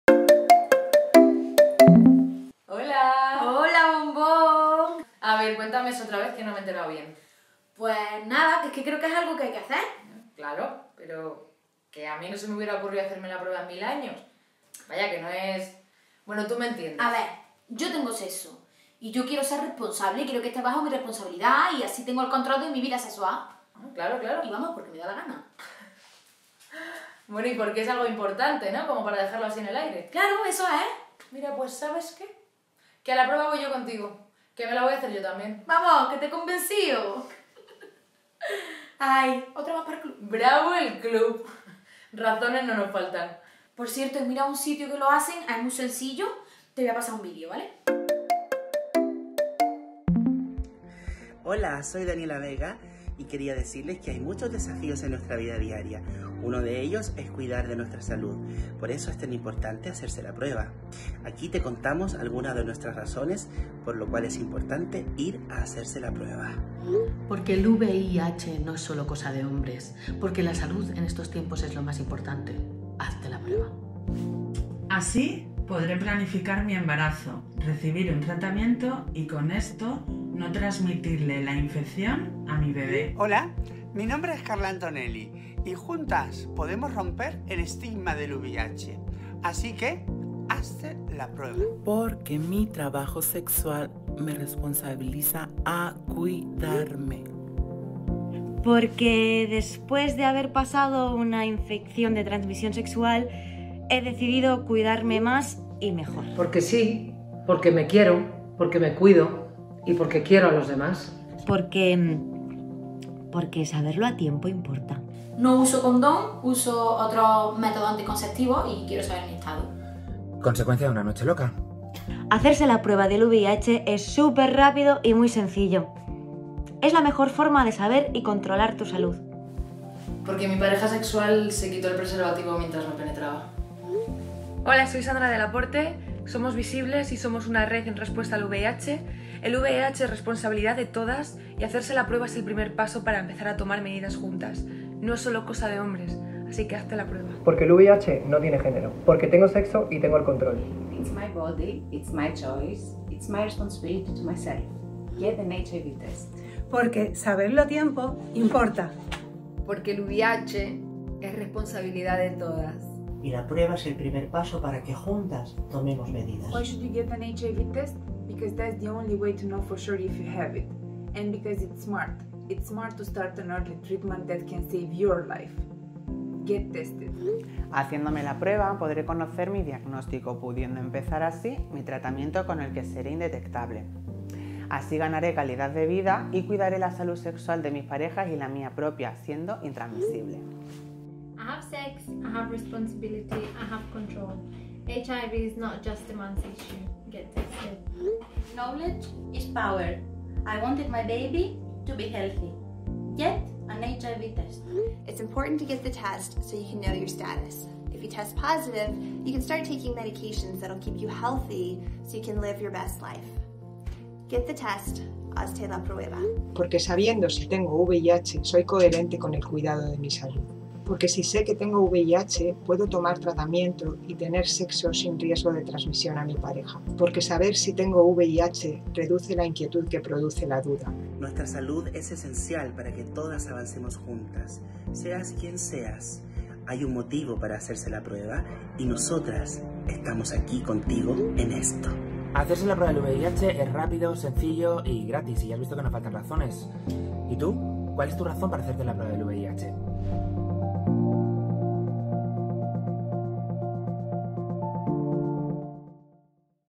¿CUÁLES SON TUS RAZONES PARA HACERTE LA PRUEBA DEL VIH?
Las mujeres tenemos sexo y hablamos de sexo. Da igual la edad, da igual la procedencia. Por eso, también entre nosotras hablamos de VIH.

Hacernos la prueba es una forma más de cuidar nuestra salud sexual. Hay muchos desafíos en nuestra vida diaria y podemos afrontarlos ¡juntas!.
“Nuestra salud es esencial para que todas avancemos juntas. Seas quién seas, hay un motivo para hacerse la prueba y nosotras estamos aquí, contigo en esto”, Daniela Vega, actriz y embajadora de RAZONES.